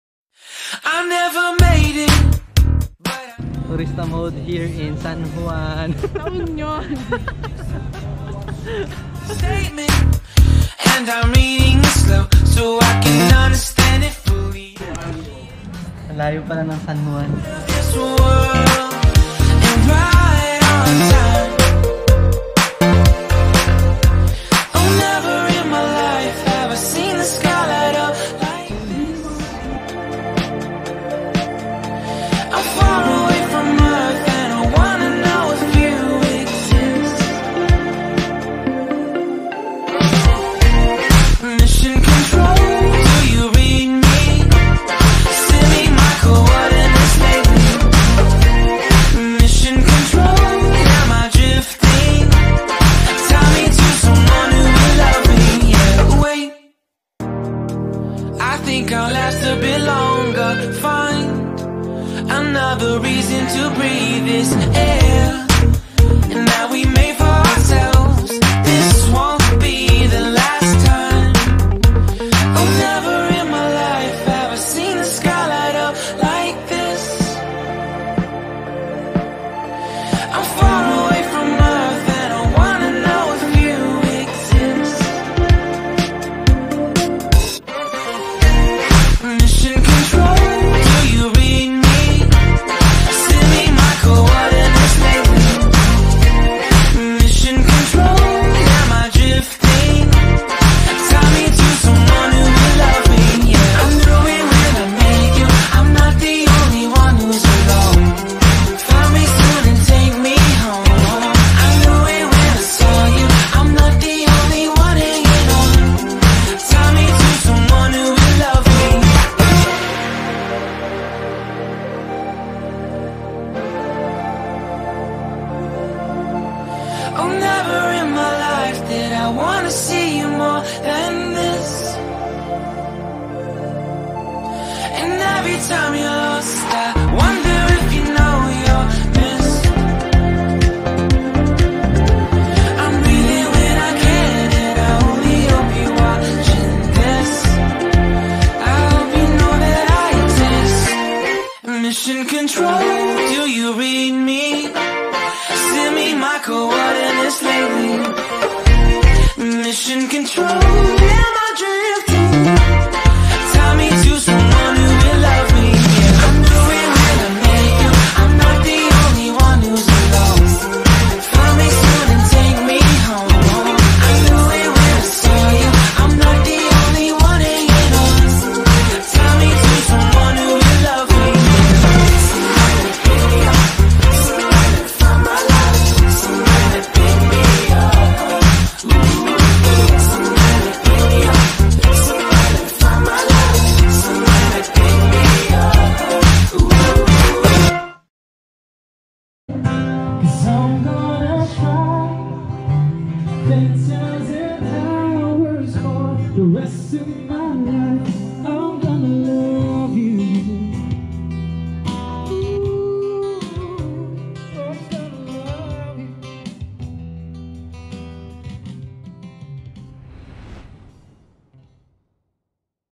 San juan La union
San Juan Surf Resort (hotel 2500php per night)
The Coffee Library (breakfast 190php per head)
Amare La Cucina ( pizza 520php spagetti 380)